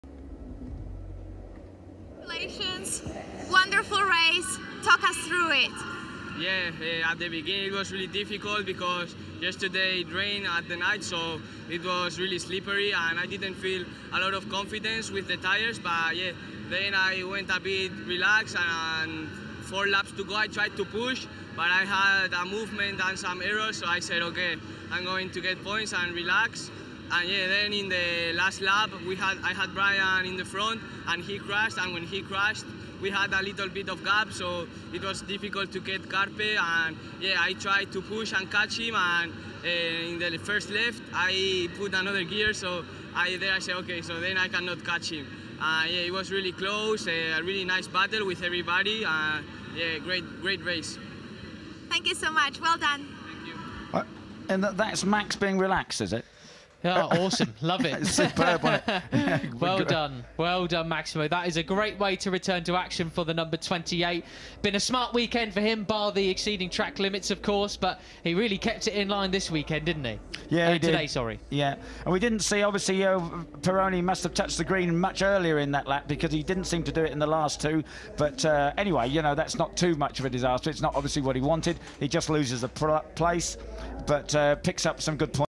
Máximo Quiles 🇪🇸 P2 Interview Austria 2024 Race 2 Red Bull Rookies Cup
Máximo Quiles' interview after finishing race 2 at the Red Bull Ring in P2. Red Bull Rookies Cup Race 2 Austria 2024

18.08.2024

©Red Bull MotoGP Rookies Cup